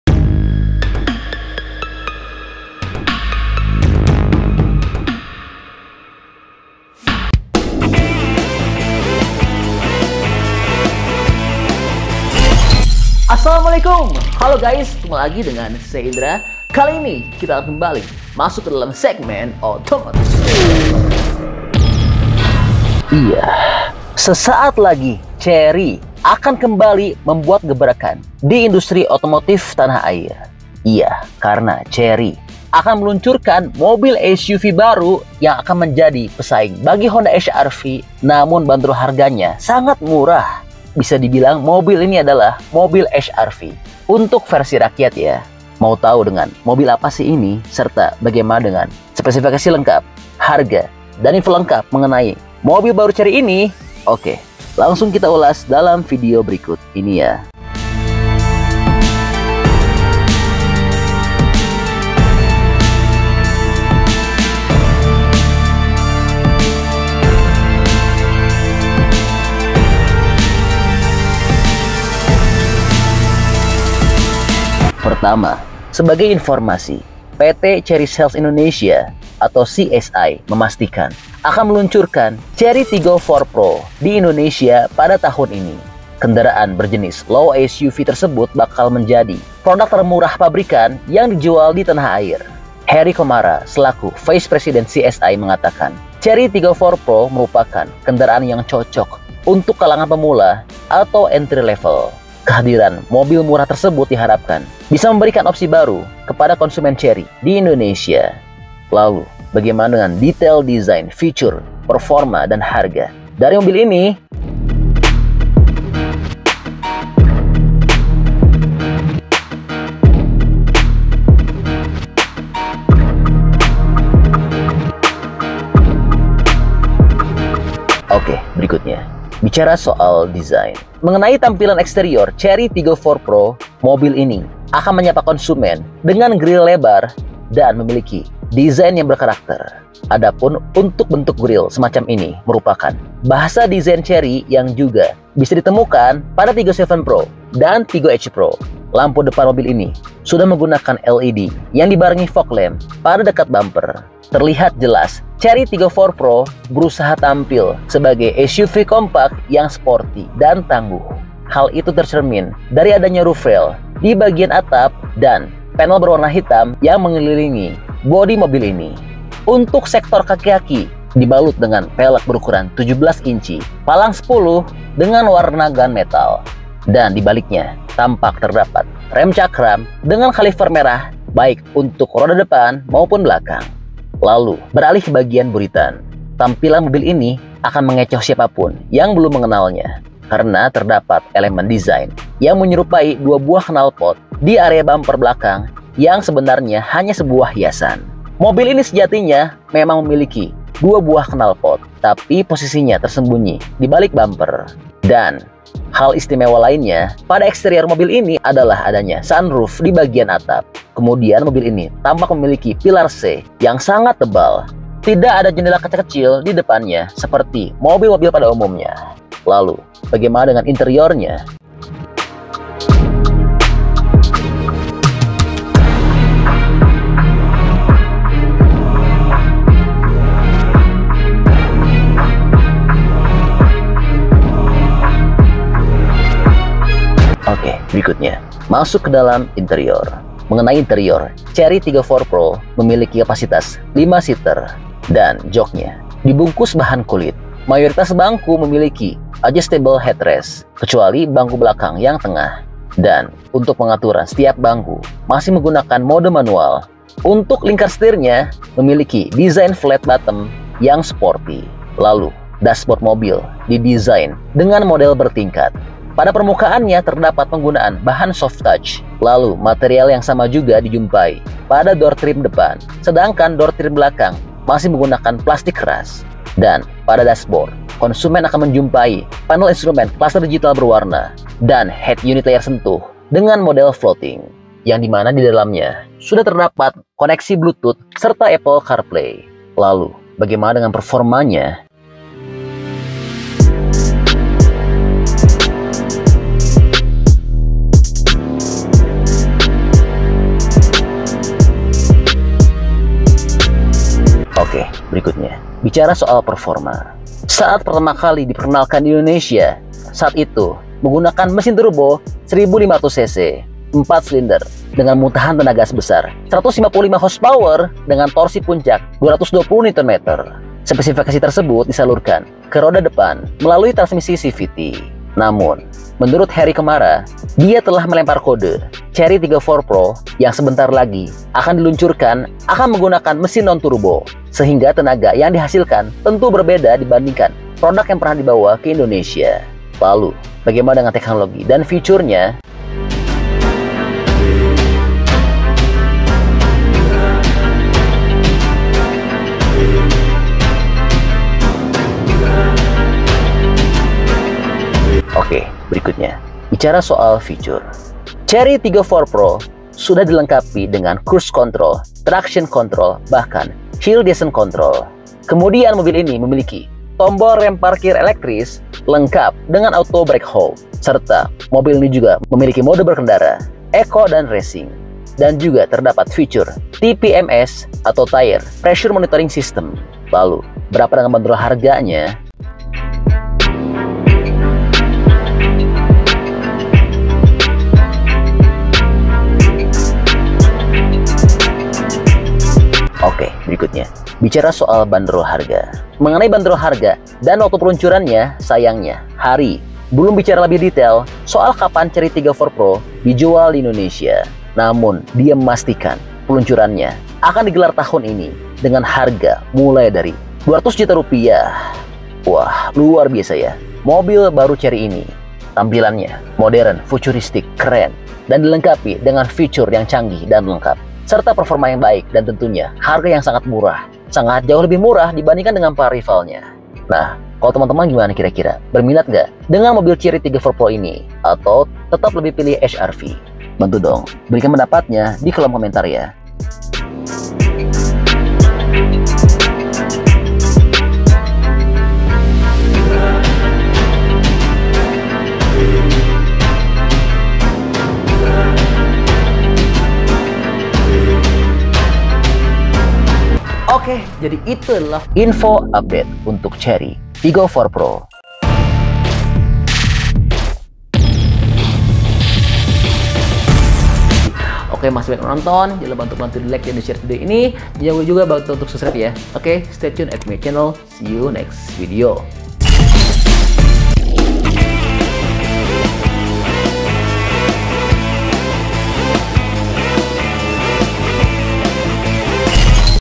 HRV MURAH VERSI RAKYAT SEGERA MELUNCUR DI INDONESIA! Siapkan Uang Kamu❗️
Chery akan segera merilis mobil terbarunya di Indonesia yang akan menjadi lawan tangguh bagi Honda HRV dan Hyundai Creta. Mobil baru Chery tersebut adalah Chery Tiggo 4 Pro. Dan mobil Chery Tiggo 4 Pro juga akan bersaing dengan Grand Vitara serta Wuling Alvez yang saat ini sudah dipasarkan di Indonesia. Dan saat ini Chery Tiggo 4 Pro sudah uji jalan di Indonesia!

Tampilan desain Chery Tiggo 4 Pro sangat modern, futuristik, dan sporty.

Chery Tiggo 4 Pro juga dilengkapi fitur yang lengkap, canggih, dan mewah. Performa mesin Chery Tiggo 4 Pro juga sangat baik.

Lalu berapa harga dari Chery Tiggo 4 Pro?
Bagaimana dengan model & spesifikasi lengkap Chery Tiggo 4 Pro?
Kapan Chery Tiggo 4 Pro masuk dan dipasarkan di Indonesia?

Selengkapnya tonton aja di video berikut, jangan di skip² ya nontonnya supaya gak ada info yang terlewat.

Terima kasih.
Selamat menonton!

Segment:
00:00 Intro
01:14 Chery Tiggo 4 Pro - Siap Rilis Di Indonesia!
02:07 Chery Tiggo 4 Pro - Desain
03:57 Chery Tiggo 4 Pro - Interior
05:04 Chery Tiggo 4 Pro - Performa
05:55 Chery Tiggo 4 Pro - Fitur
06:33 Chery Tiggo 4 Pro - Harga